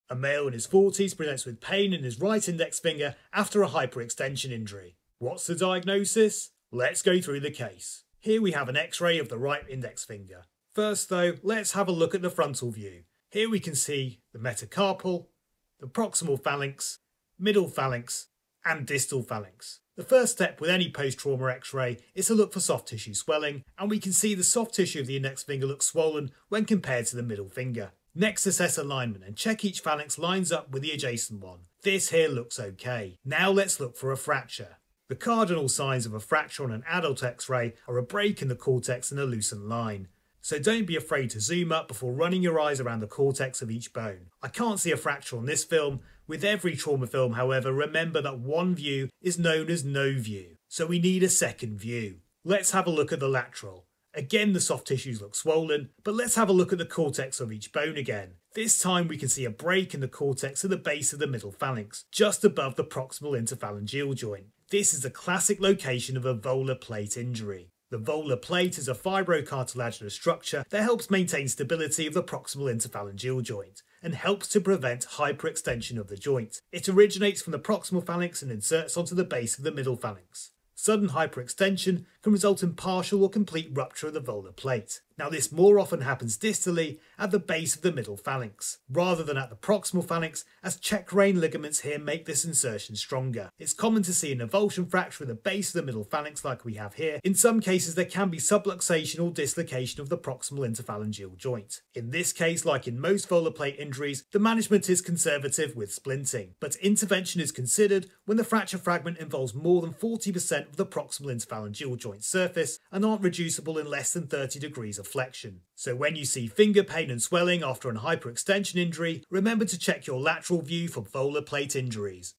Finger pain post hyperextension: how to assess volar plate injuries on X-Ray
A male in his 40s presents with finger pain and swelling after a hyperextension injury. What does the X-Ray show?

VOLAR PLATE AVULSION FRACTURE
👨🏽‍💻Did you spot it? There is a small fragment of bone at the base of the middle phalanx on the volar aspect

👨🏽‍💻This is a common finding with an avulsion fracture at the volar plate, usually the result of a hyperextension injury

👨🏽‍💻The fracture can be much more subtle than this one so make sure you scrutinise this area of the finger on the lateral view! It commonly won’t be seen on the frontal view